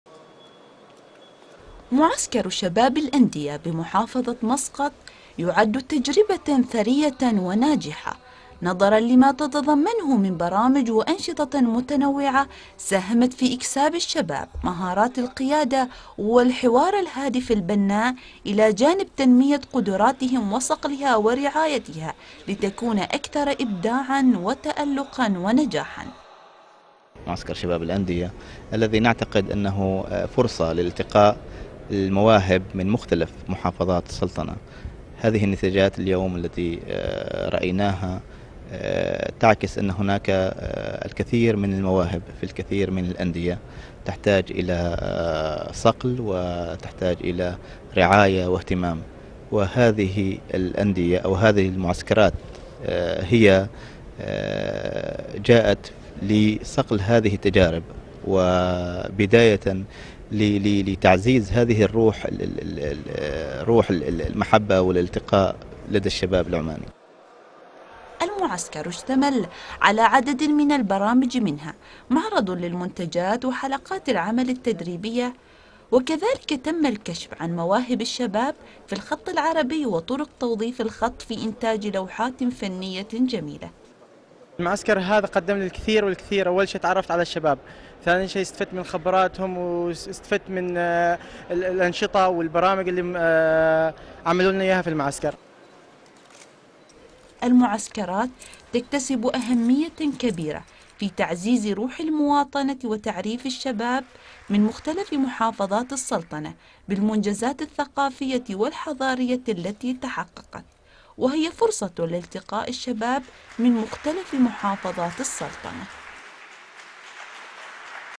اختتام فعاليات أنشطة معسكر شباب الأندية بمحافظة مسقط
تقرير/ محمد بن صالح البلوشي
قراءة / ثريا بنت سعيد الذهلية